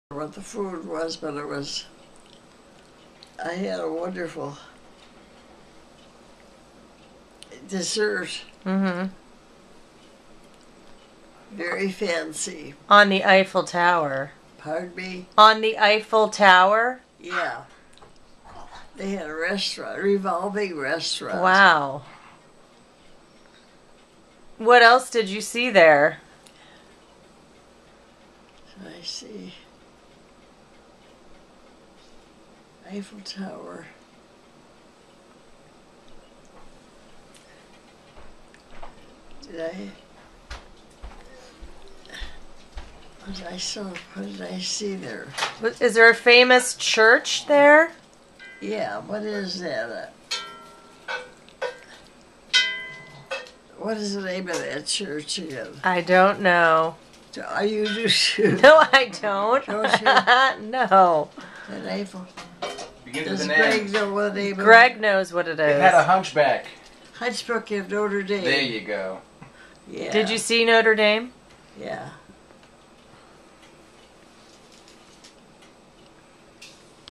Aunt T talks of dessert in Paris
Aunt T - answers questions about having dinner on the Eiffel Tower and going to Mass at Notre Dame